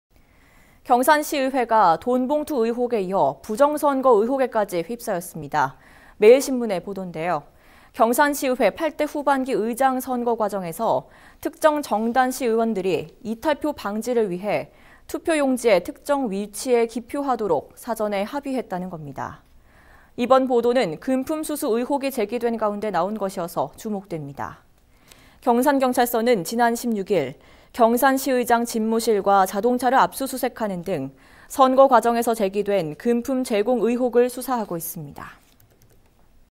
'경산시의회 돈봉투에 부정선거 의혹까지'
경산시의회가 돈봉투 의혹에 이어 부정선거 의혹에까지 휩싸였습니다. 매일신문의 보도인데요, 경산시의회 8대 후반기 의장 선거과정에서 특정 정당 시의원들이 이탈표 방지를 위해 투표용지의 특정 위치에 기표하도록 사전에 합의했다는 건데, 이번 보도는 금품 수수 의혹이 제기된 가운데 나온 것이어서 주목됩니다.